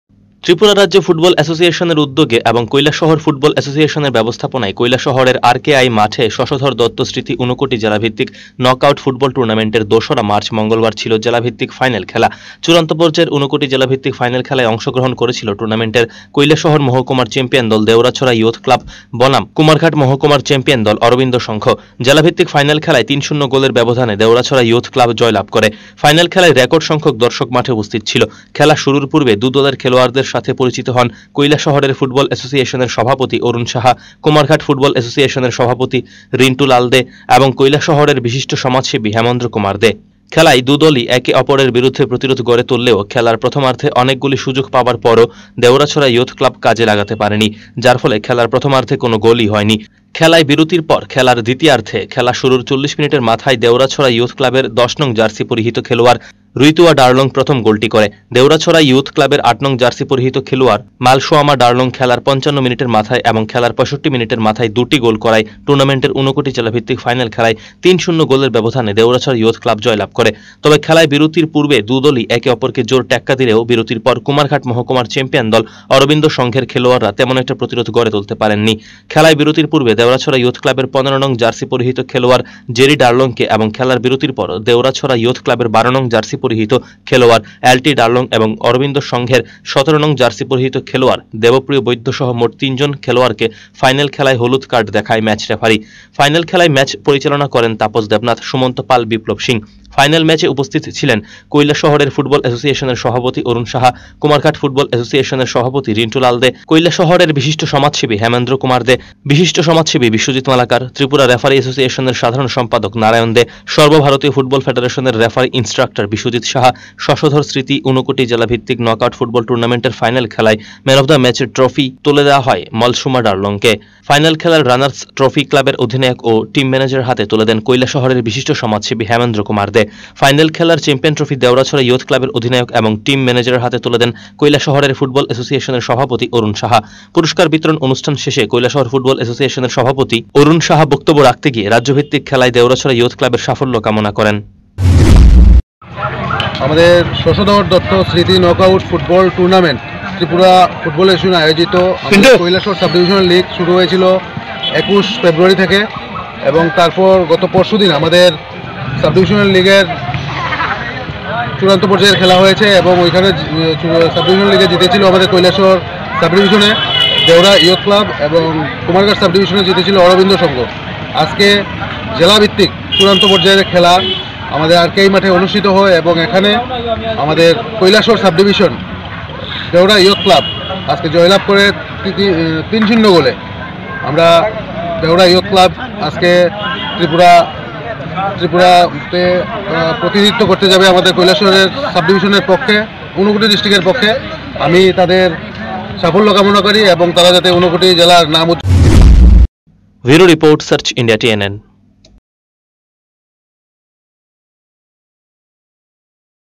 চ্যাম্পিয়ন দেওরাছড়া ইয়থ ক্লাব
ত্রিপুরা রাজ্য ফুটবল এসোসিয়েশনের উদ্যোগে এবং কৈলাসহর ফুটবল এসোসিয়েশনের ব্যবস্থাপনায় কৈলাসহরের আর.কে.আই মাঠে শশধর দত্ত স্মৃতি ঊনকোটি জেলা ভিত্তিক নক আউট ফুটবল টুর্নামেন্টের দুসরা মার্চ মংগলবার ছিল জেলা ভিত্তিক ফাইনাল খেলা।